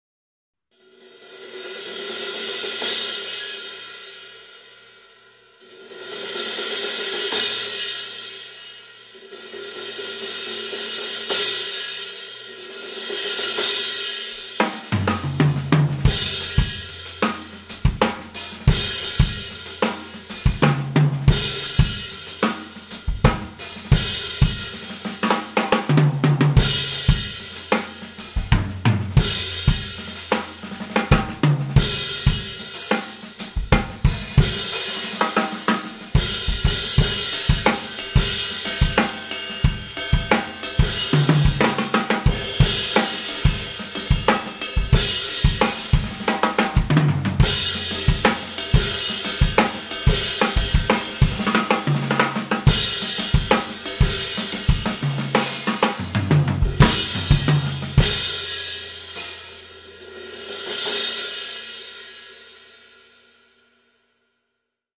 ZIldjian 19" K Dark Thin Crash Cymbal - Brilliant - 1486g (K0013-1110524H)
ZIldjian 19" K Dark Thin Crash Cymbal - Brilliant available from myCymbal.com

Other gear used in this video:
• Cymbals (From drummer's L to R): Zildjian 14" K Constantinople Light Hi-Hats, Zildjian 22" A Avedis Dark Ride Cymbal
• Snare: Yamaha 14x5.5 Recording Custom Aluminum
• Drumheads: Remo Clear Powerstroke 3, Remo Coated Ambassadors
• Sticks: Vic Firth 5A